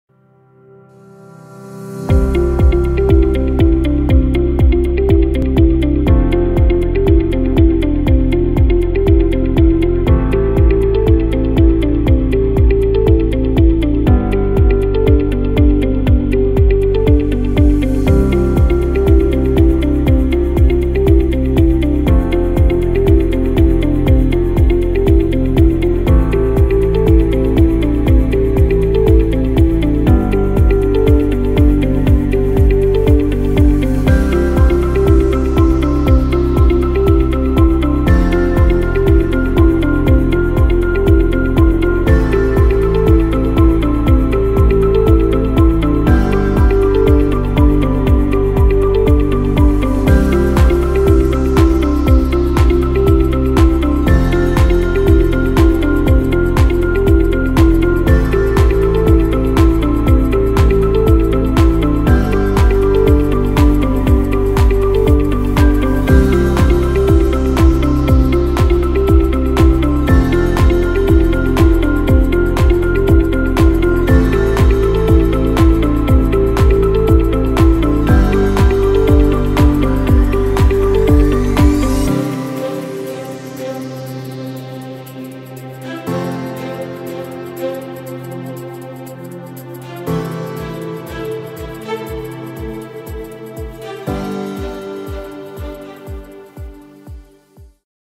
Bolette - Fred Olsen Cruise lines New Flagship - Cruise Ship Tour Video
Fred Olsen's Bolette - video of key areas taken onboard the Maiden Voyage of the new Fred Olsen Flagship in August 2021. This ship was bought from Holland America Line and refurbished during the pause in worldwide cruise sailings in 2020/2021.